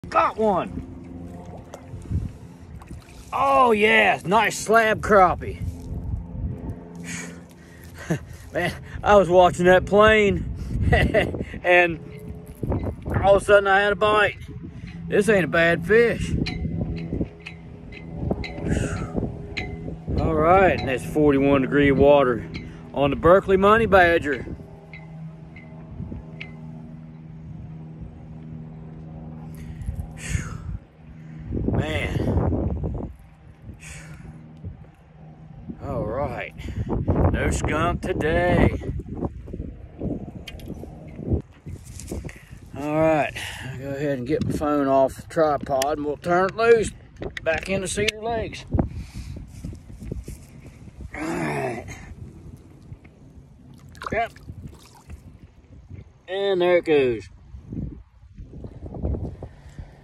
Crappie on a Money Badger at Cedar Lakes, in 41° water!
Fishing at Cedar Lakes and catching a nice Crappie on a Berkley Money Badger in 41° water!